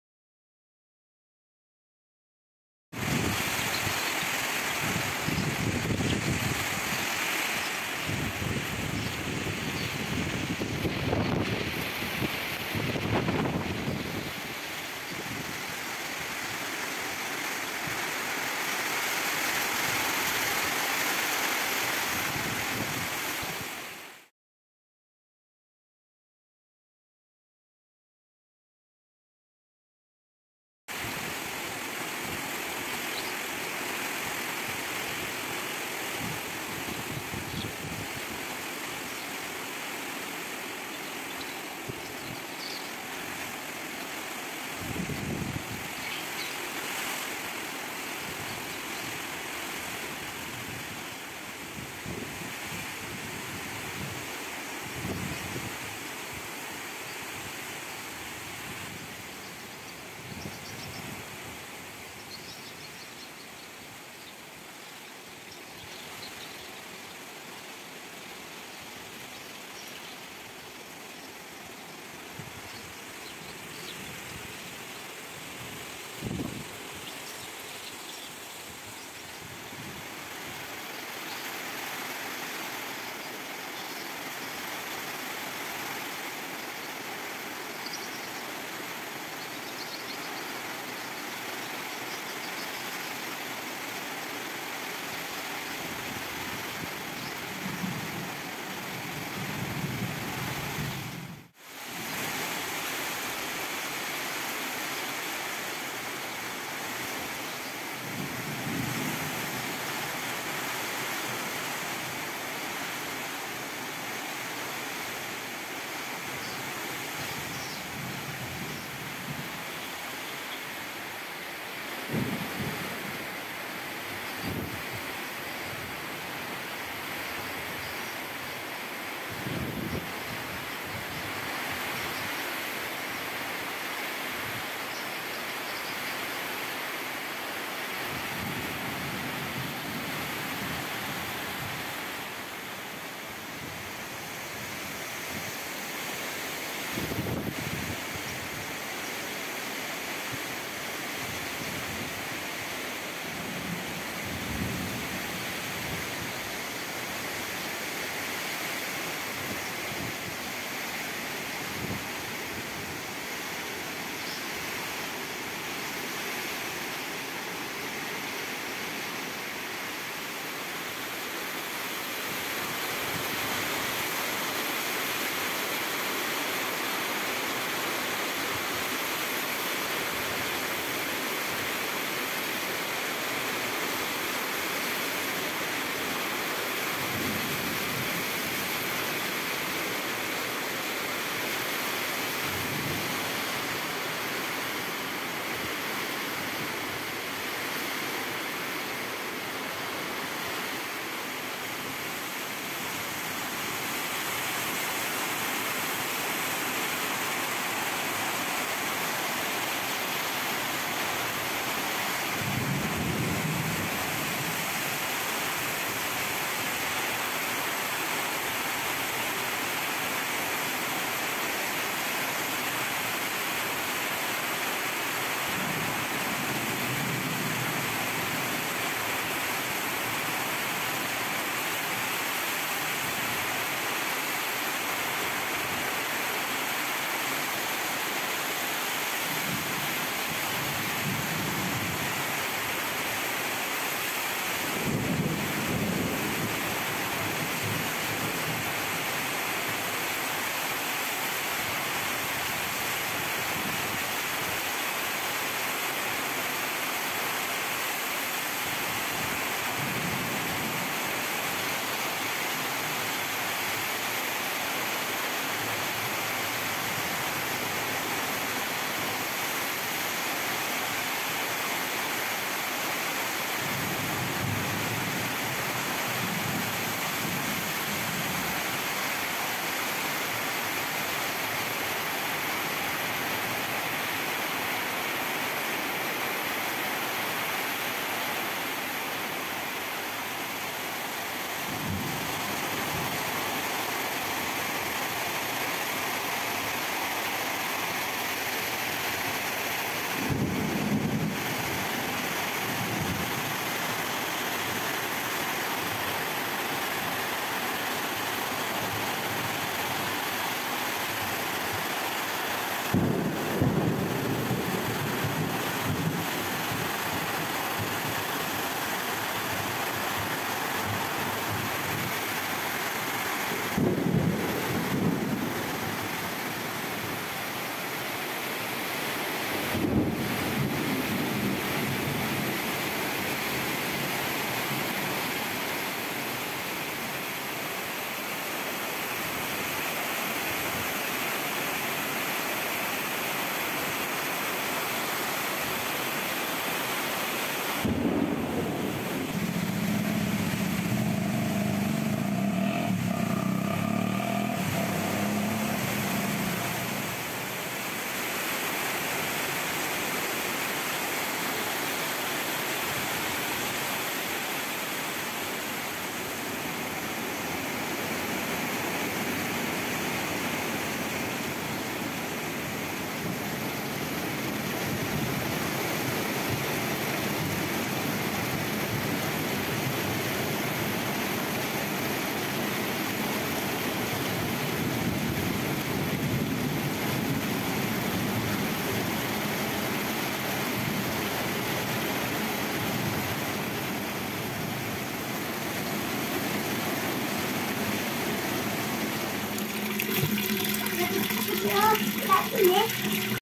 PENAMPAKAN DESA BELANTARAYA SAAT HUJAN KILAT BERPETIR 2022
Desa BELANTARAYA merupakan salah satu Desa yang Ada di Kecamatan Gaung kabupaten Indragiri Hilir Provinsi Riau. Desa BELANTARAYA yang berada di pedalaman pulau Sumatera ini merupakan salah satu Desa yang besar dan maju, pasar desa BELANTARAYA tertata rapi, banyak gedung tinggi,. kita akan lihat pemandangan desa BELANTARAYA saat hari hujan, angin kencang, berkilat dan berpetir  di siang hari.